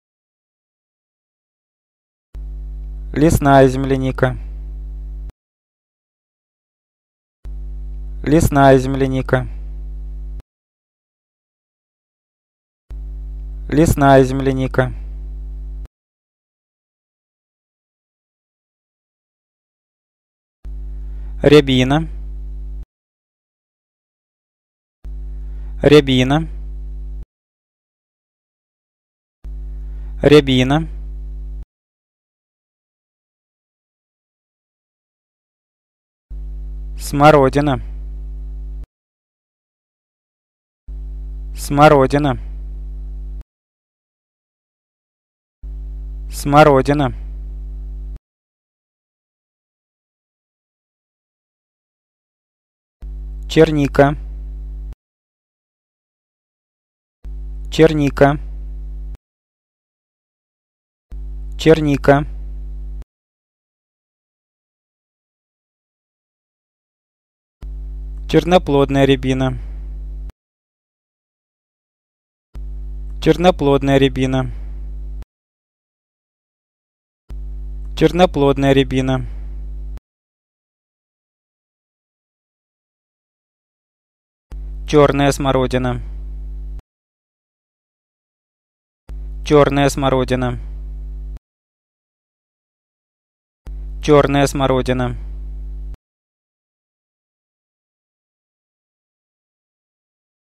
Russian Phrases and words / berries (161) / Russian language
Russian Phrases and words / berries
rowan
black chokeberry
red currant
black currant
blueberry
wild strawberry